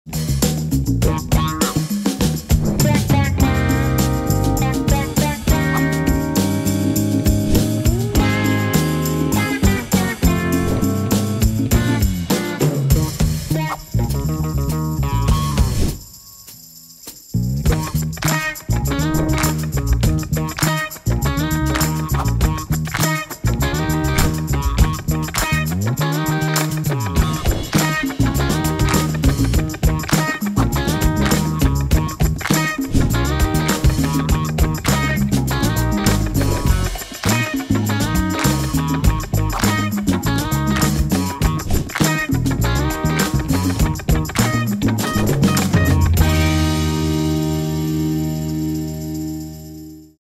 Viikon 9 ruokalista - Katso nopeat vinkit! | K-Ruoka
00:00 Intro
00:03 Kanacurry
00:11 Tomaattinen jauhelihakeitto
00:19 Nyhtökaurakäristys
00:28 Lime-kookoskana
00:37 Uunilohipasta

Nämä ja muut herkulliset reseptit K-Ruoka-mobiilisovelluksesta. Lataa sovellus